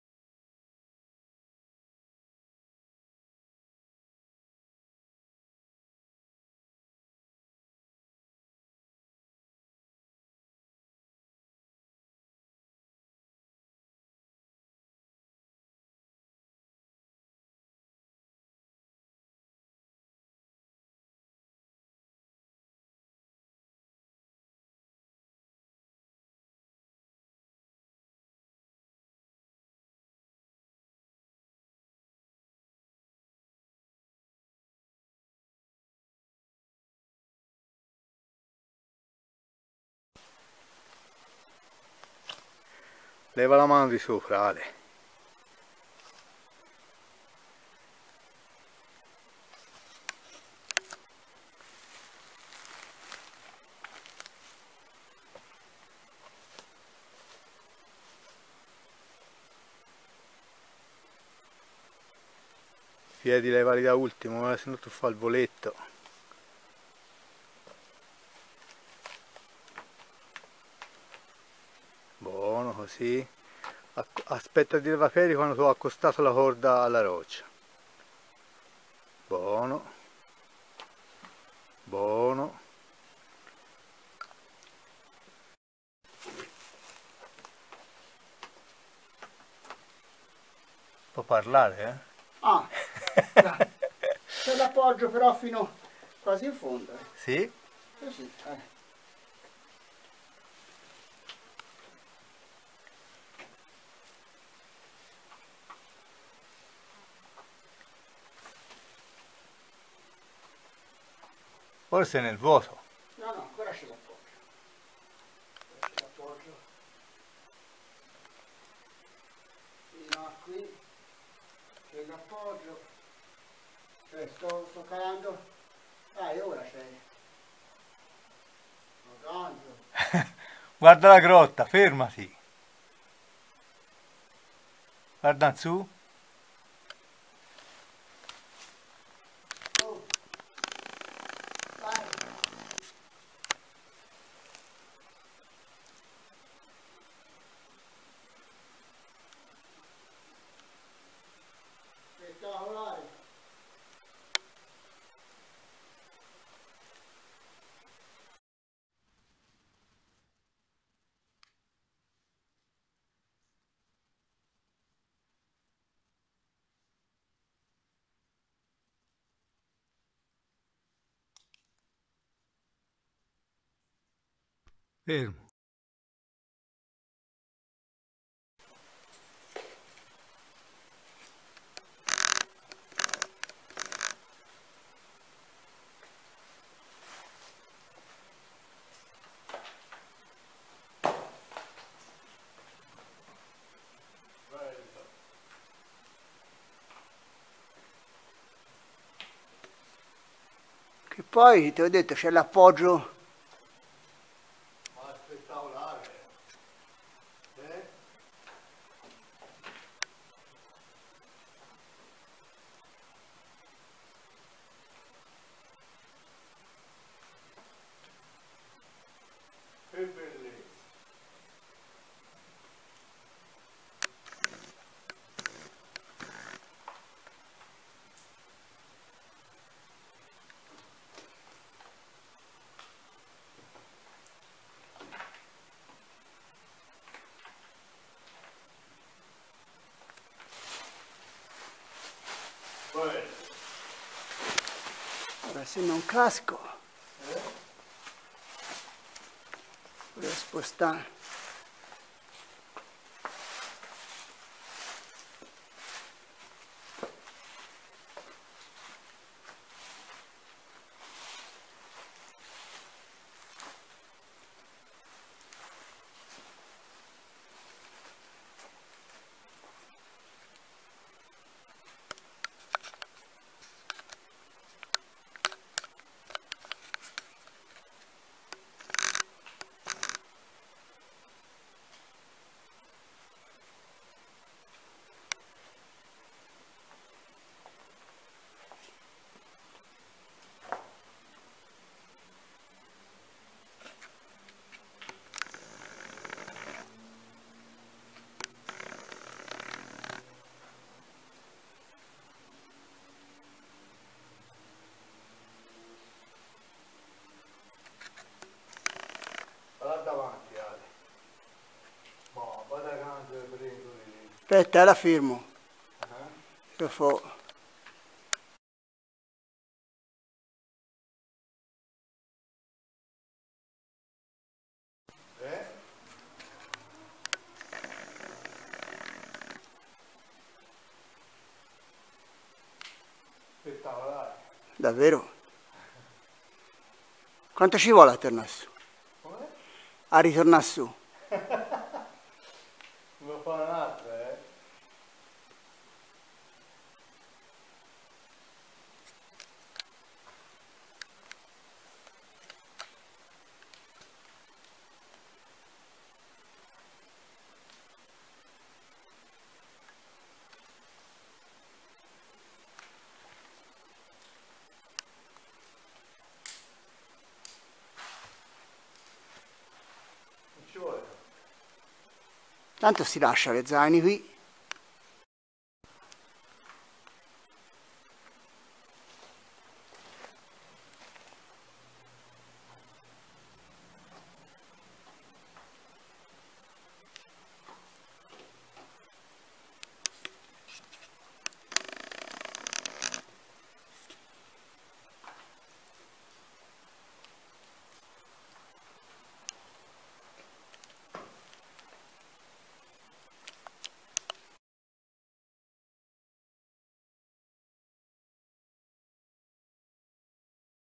Calata nella grotta della Piella 20 Settembre 2020
Calata nella grotta della Piella dall'ingresso alto